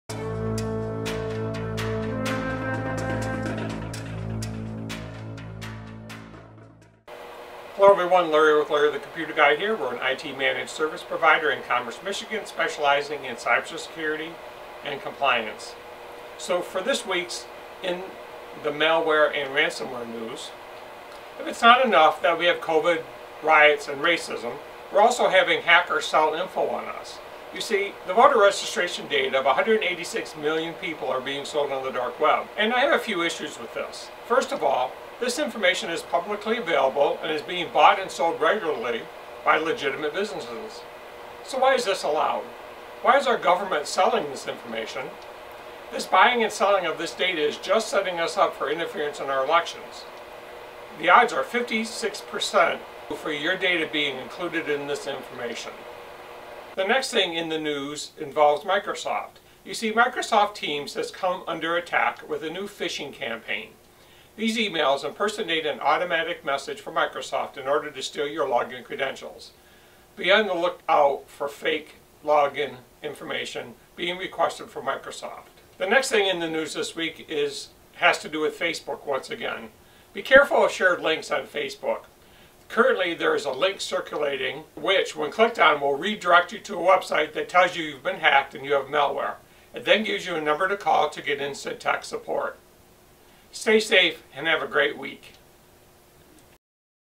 Vlog 97
Hackers selling voter registration info and malware scams to be aware of...